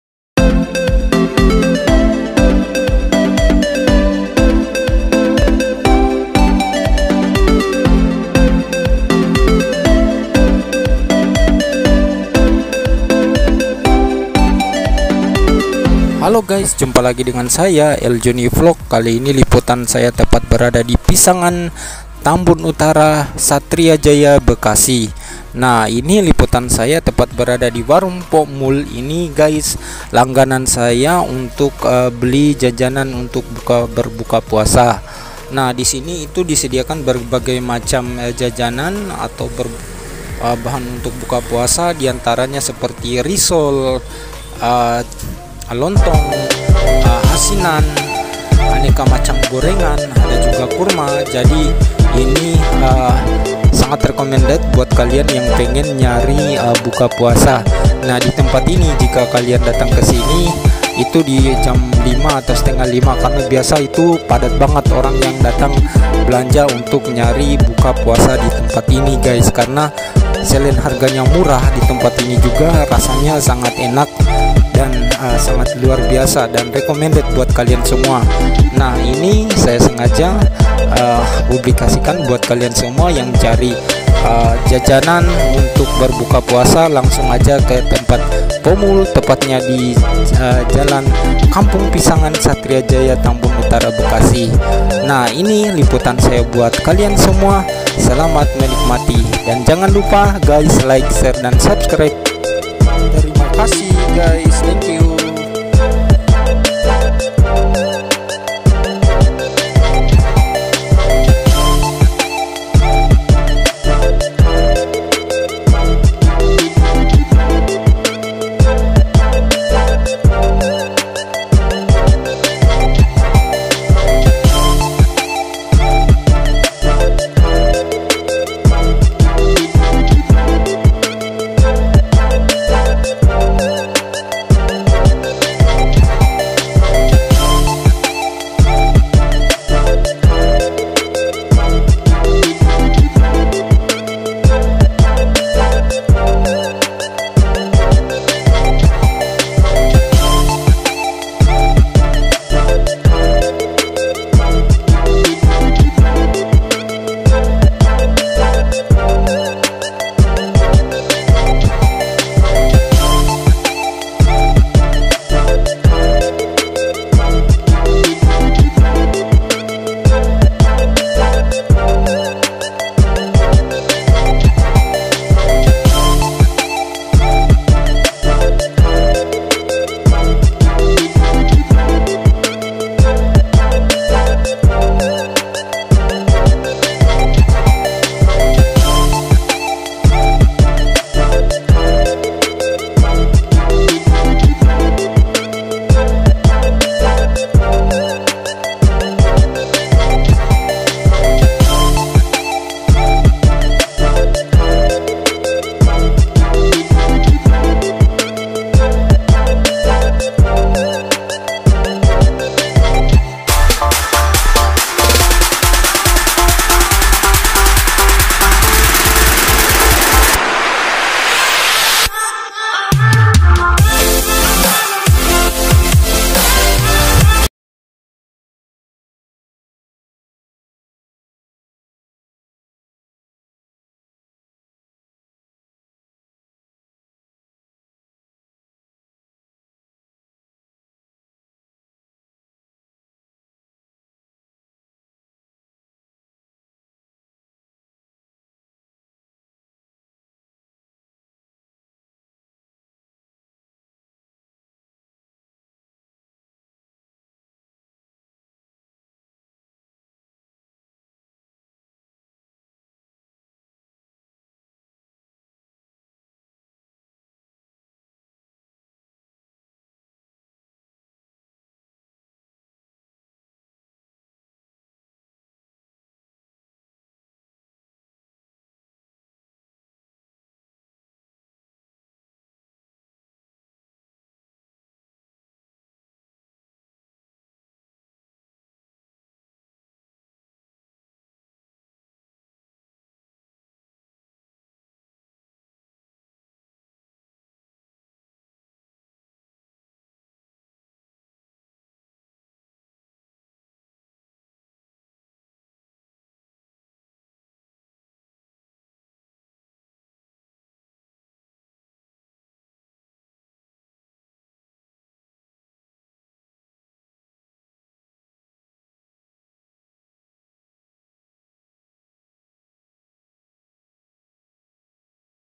Warung Mpo Mul menjual Suguhan Buka Puasa
Halo gays apa kabar kalian semua, semoga sehat sehat wal afiat yah amin...
Di bulan puasa ini liputan saya tepat berada di kampung Pisangan Satria Jaya Tambun Utara Bekasi tepatnya di warung Mpo Mul.
Disini warung langganan saya untuk mencari suguhan buka puasa selain cita rasanya enak juga harganya sangat murah dan ini sangat recomendet buat kalian yang pengen nyari jajanan aneka gorengan, risol, lontong, asinan.